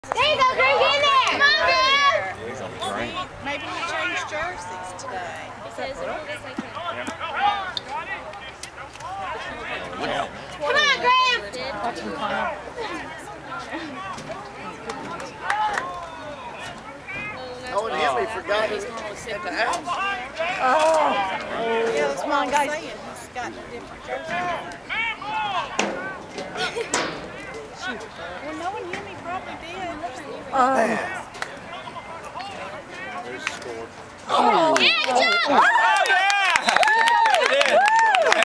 A sweep goal
Hillgrove JV Lacrosse victory over McEachern in the JV Tournament- April 18, 2009